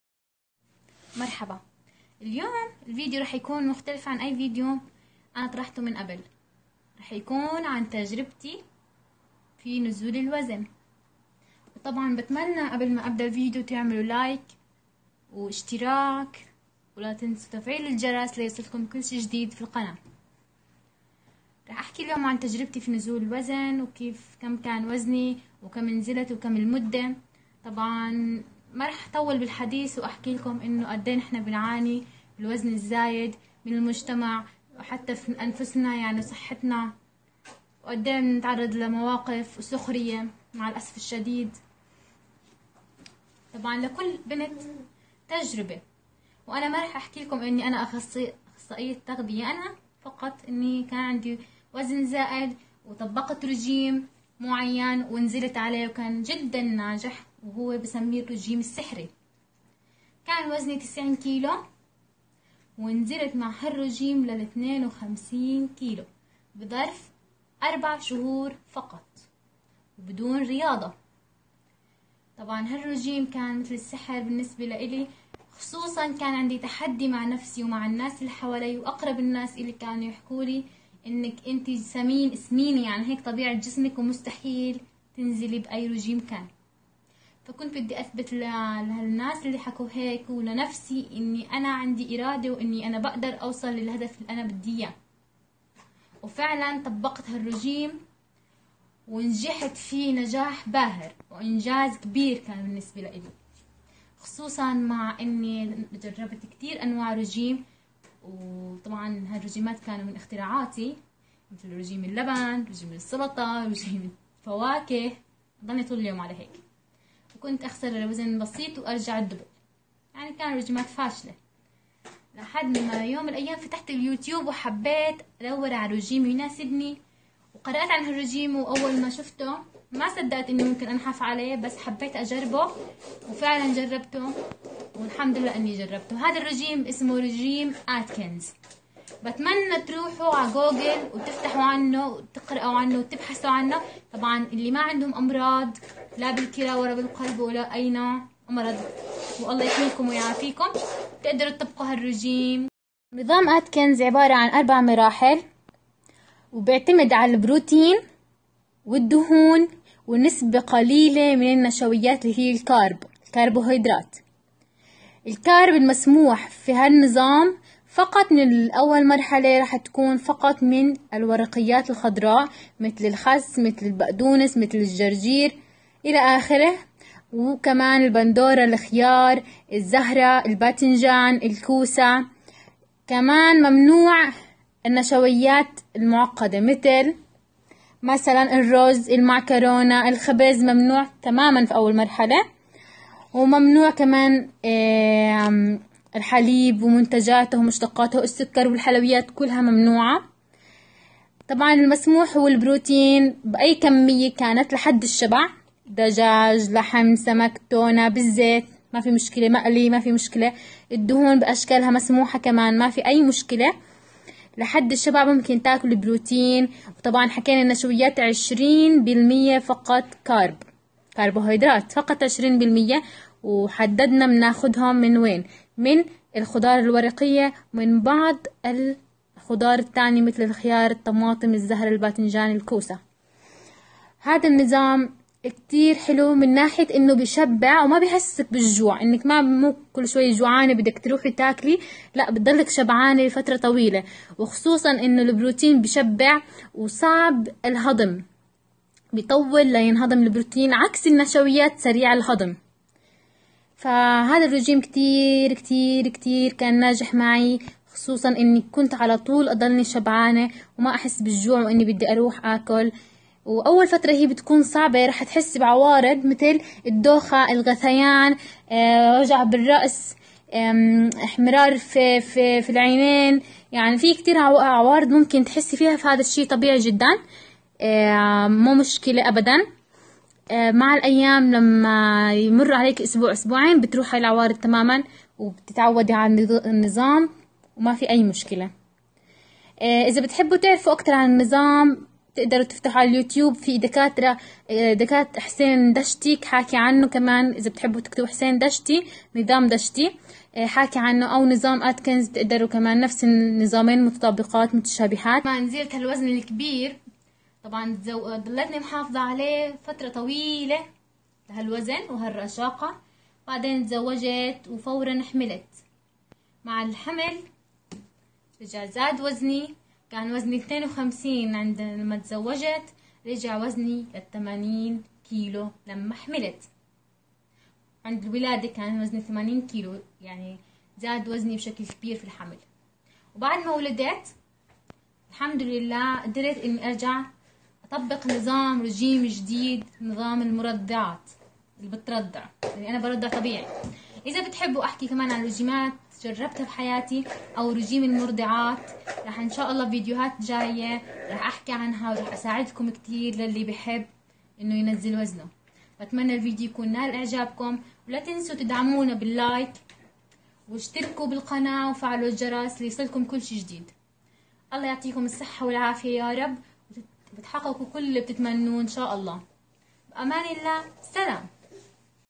كيف نحفت 37 كيلو بدون رياضة | فيديو رح يغير حياتك
رابط طلب تيشيرت الخط العربي من تصميمي عن طريق موقع امازون
انستجرامي
ahlam.designer1
فيديو لكل فتاة تعاني من الوزن الزائد
رح يغر حياتك بمجرد تغير نظام اكلك بدون حرمان
بنقدم كل مفيد وبنستنى الدعم لنستمر